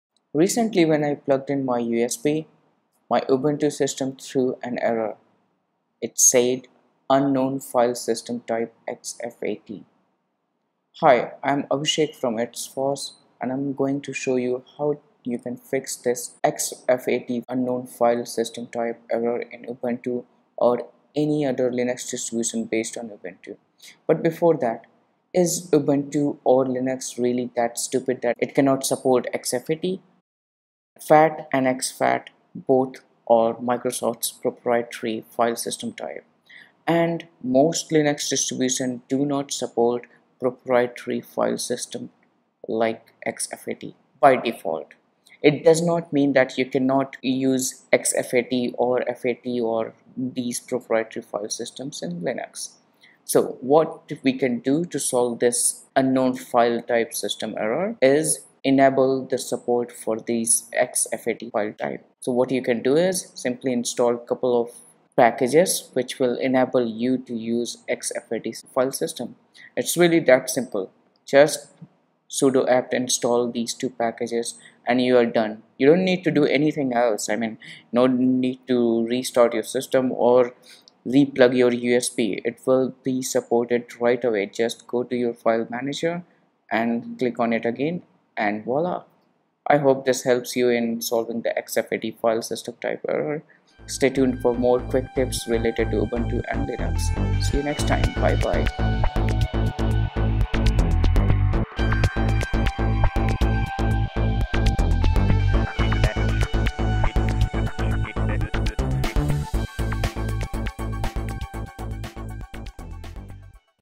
How to Mount and Use exFAT Drive on Ubuntu Linux [Quick Tip]
his quick video tutorial shows you how to enable exFAT file system support on Ubuntu and other Ubuntu-based Linux distributions. This way you won’t see any error while mounting exFAT drives on your system.